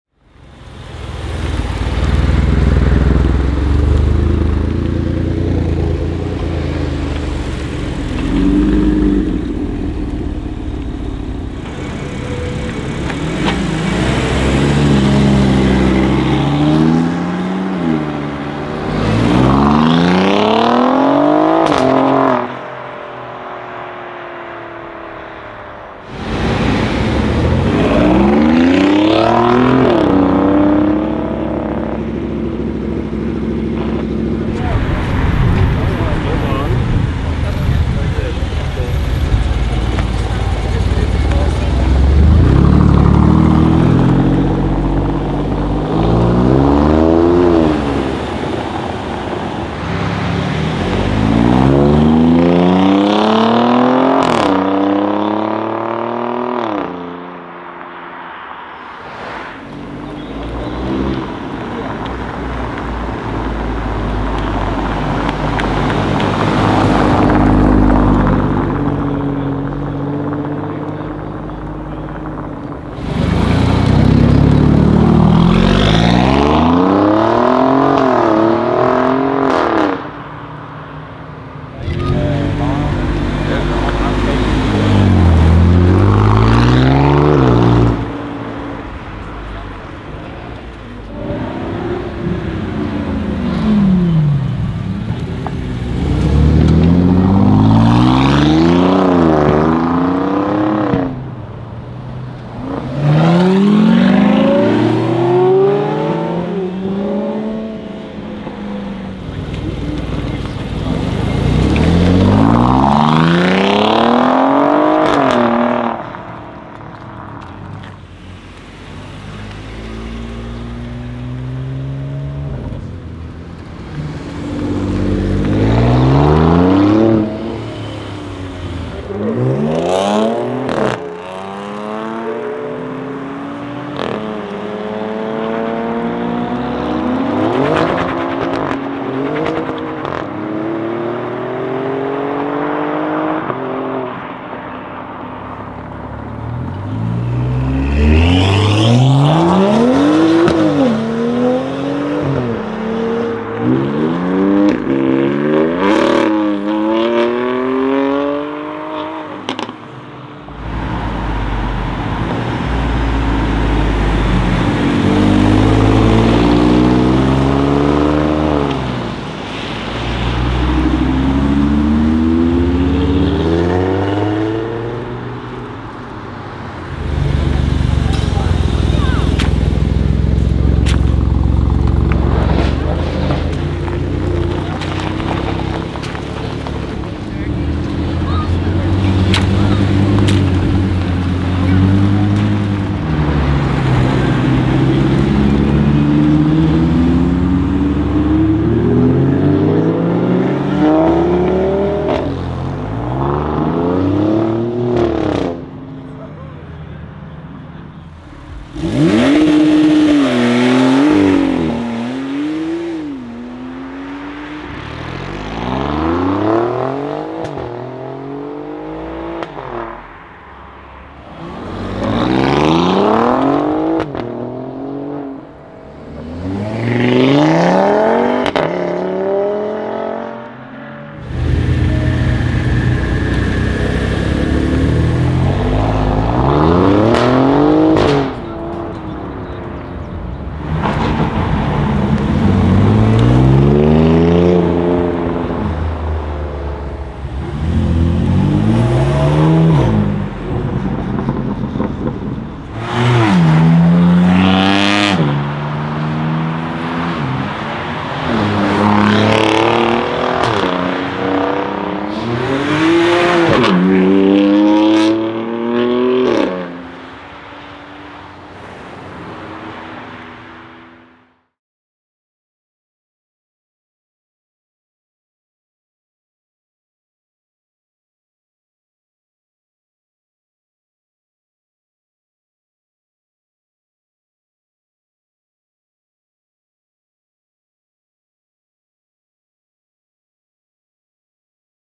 Alfa Romeo Giulia Quadrifoglio - Acceleration Sounds !
This video features a few Alfa Romeo Giulia QV's which we saw at different events in The Netherlands and Belgium. In this video you can see them making some pretty loud accelerations. The Giulia QV has a 2.9L Twin Turbo V6 engine producing 510HP and 600Nm of torque.

We saw this sportscars during the StreetGasm Autumn Drive, StreetGasm Aftermovie, Sublime Cars & Karts, Supercarsunday Assen, Haspengouw Tour, StreetGasm Cars & Sushi, GMTEC Supercarmeeting, Crème de la Car and Spring Event.